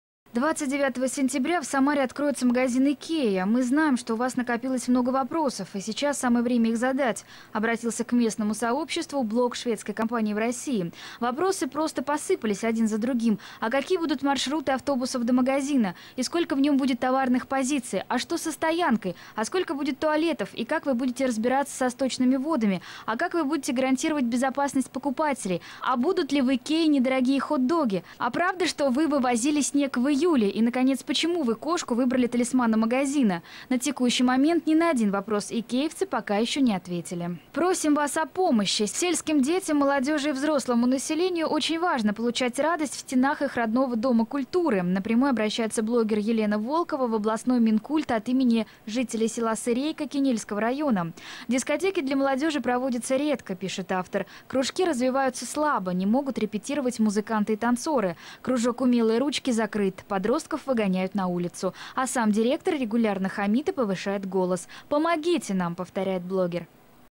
Новости блогов 12.09.11
«29 сентября в Самаре откроется магазин ИКЕА! Мы знаем, что у вас накопилось много вопросов, и сейчас самое время их задать», - обратился к местному сообществу блог шведской компании в России. А блоггер elenav0lk0va напрямую обращается в областной минкульт от имени жителей села Сырейка Кинельского района.